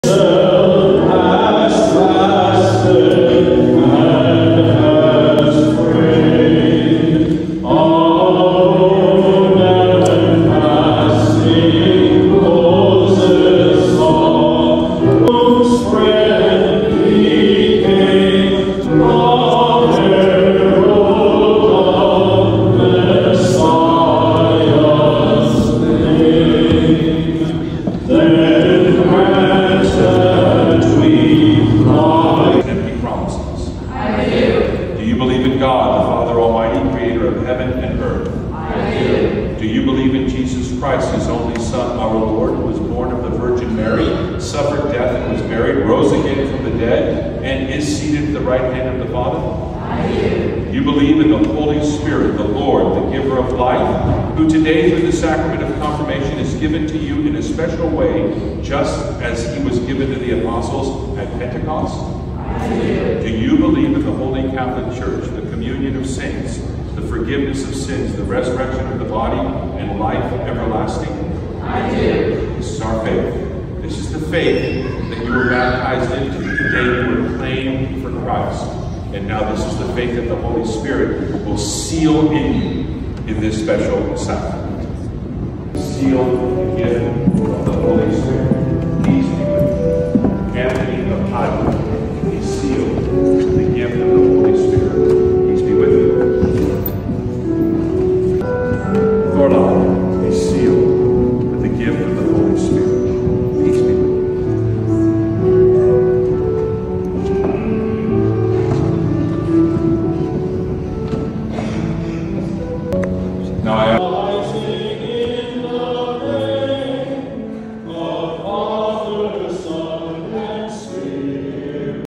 3/10/2024 My Confirmation, Holy Trinity Catholic Church
My confirmation with Vicar General Rev. James Misko.

Saint Thorlak